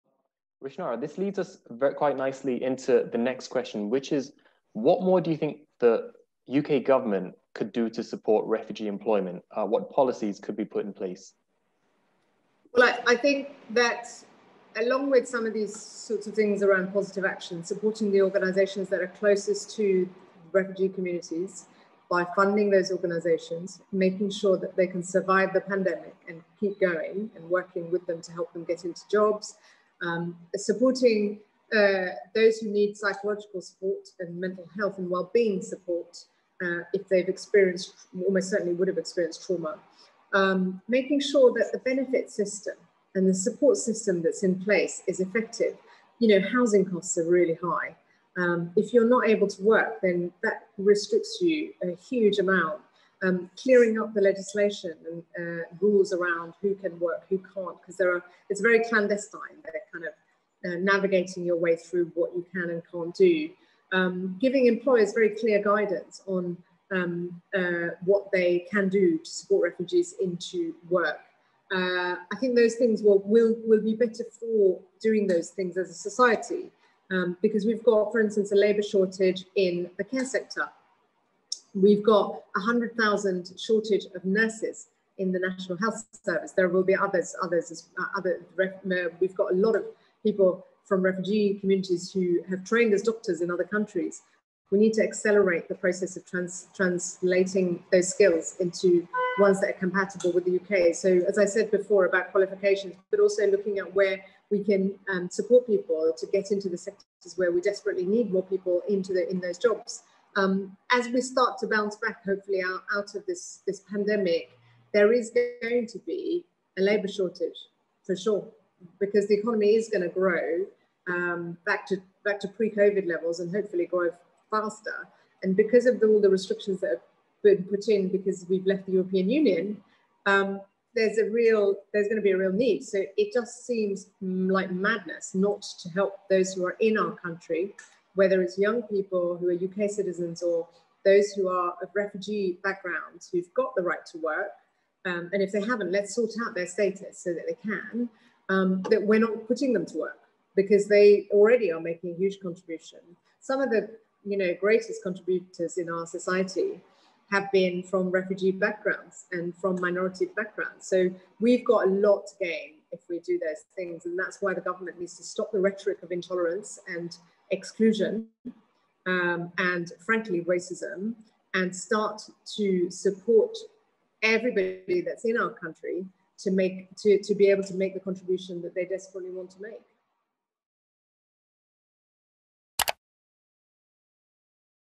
How can the UK Government Support Refugees? | Rushanara Ali MP | SonaTalks
to hire refugees today.

At Sona Circle Recruitment, we simplify the process by selecting a pool of great candidates who fit your requirements.